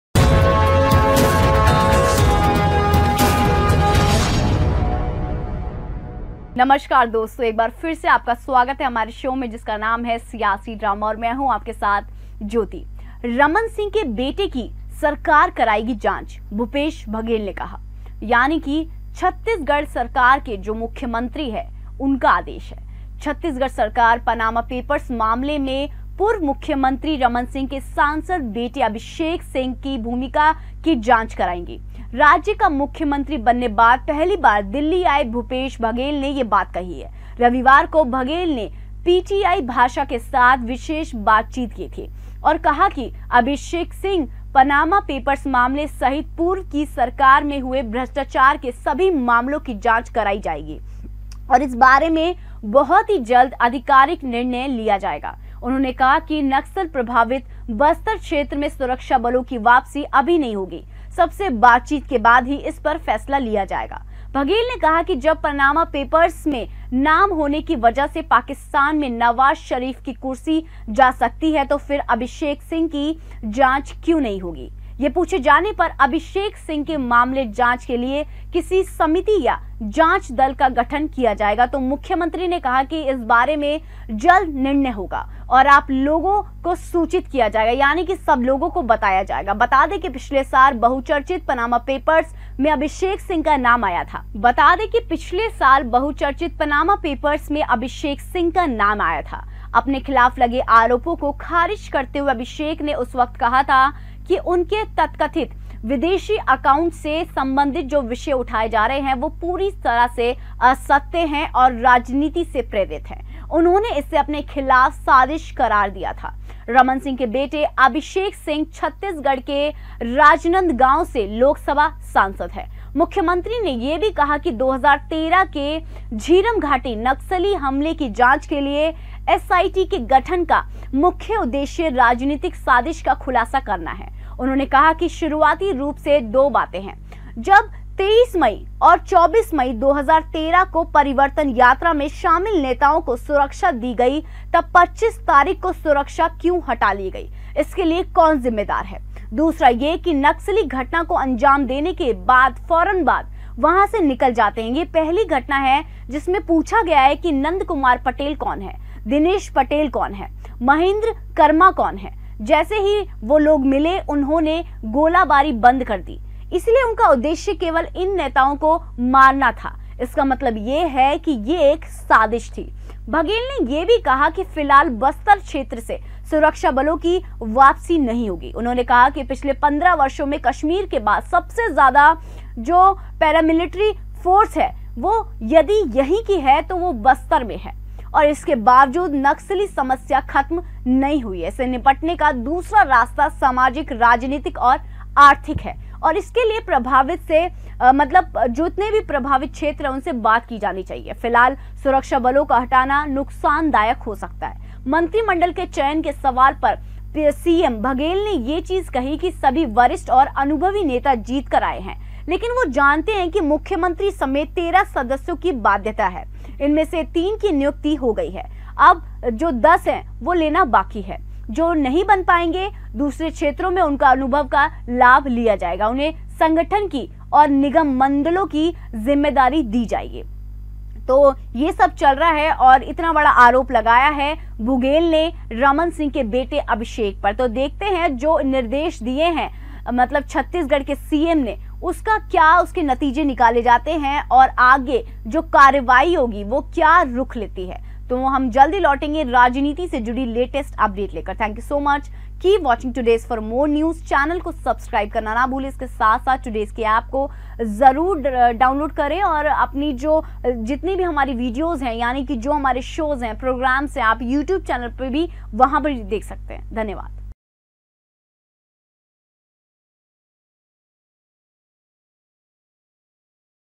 भूपेश बघेल ने चली अभिषेक सिंह के खिलाफ चाल, फसेंगे बुरी तरह //Latest News on Chhattisgarh
भूपेश बघेल ने चली अभिषेक सिंह के खिलाफ चाल, फसेंगे बुरी तरह